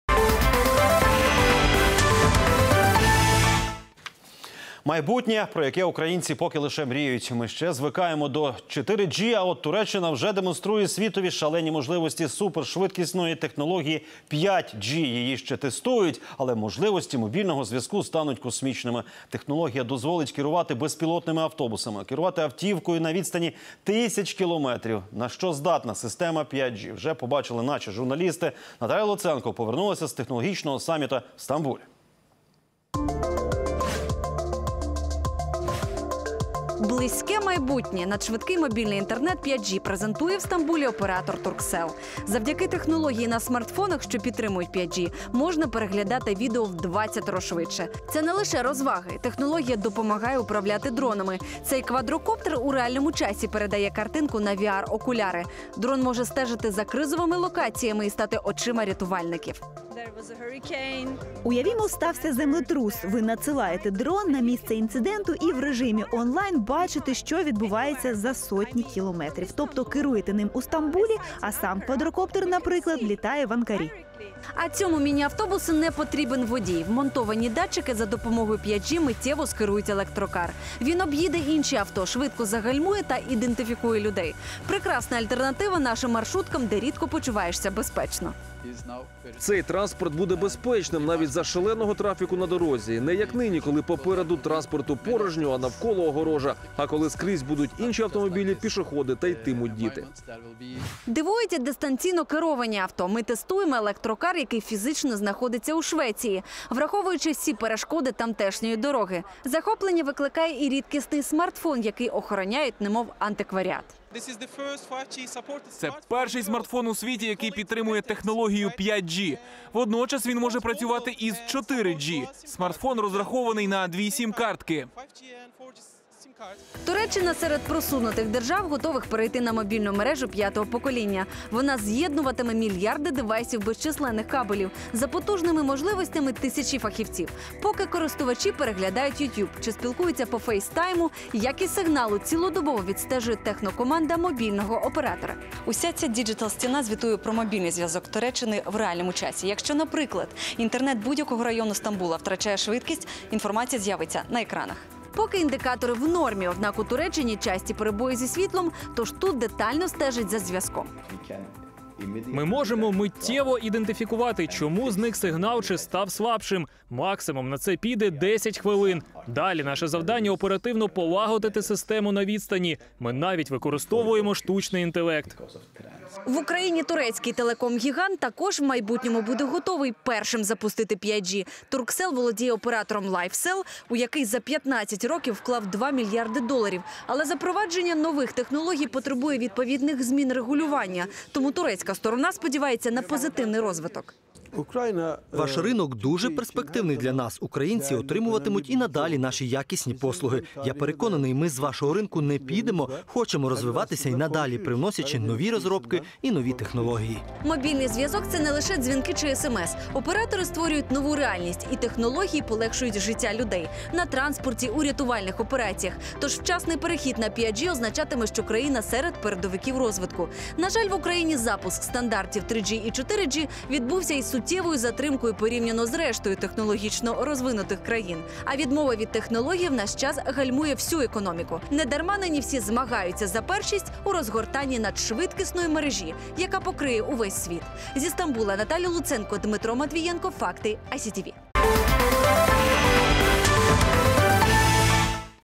Интернет 5G: на что способен и когда ждать в Украине
Пока мы еще привыкаем к 4G, Турция, уже демонстрирует миру огромные возможности суперскоростной технологий 5G.